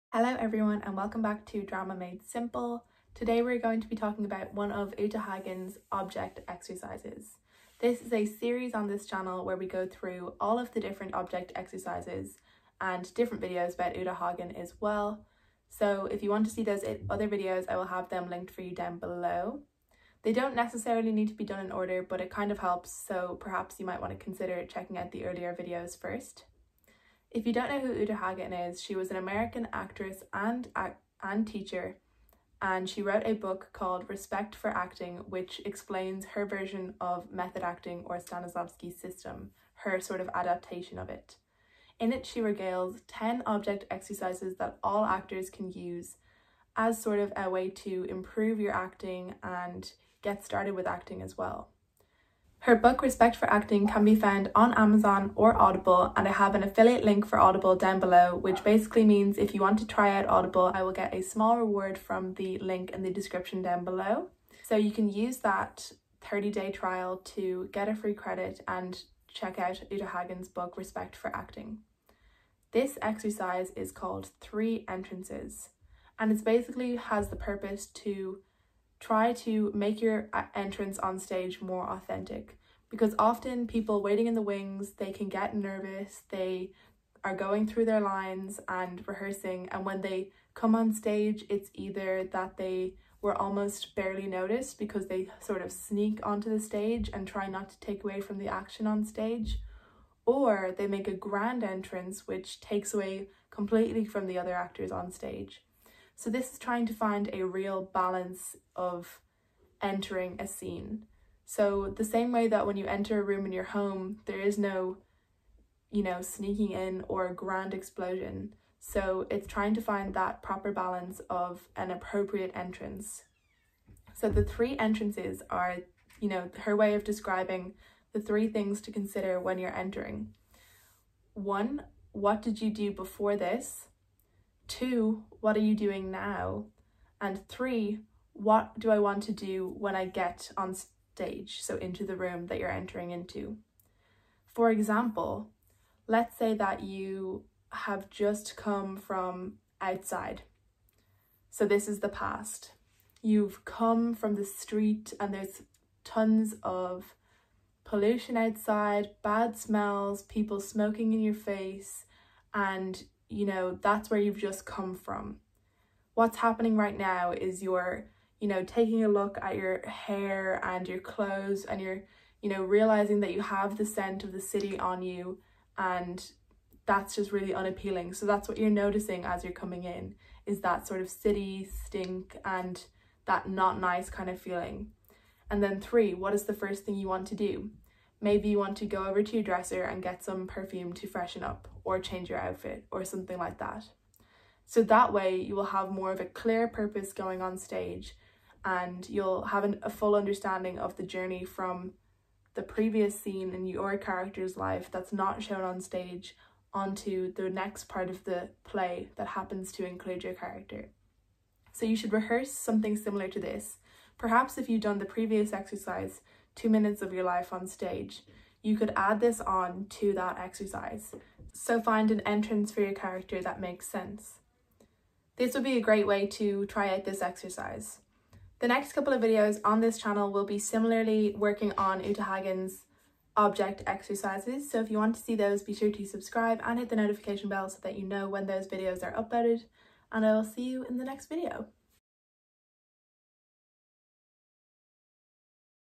How to walk on-stage | acting exercises to do at home for beginners | uta hagen object exercises
Part 2 of 10: Uta Hagen's 10 object exercises.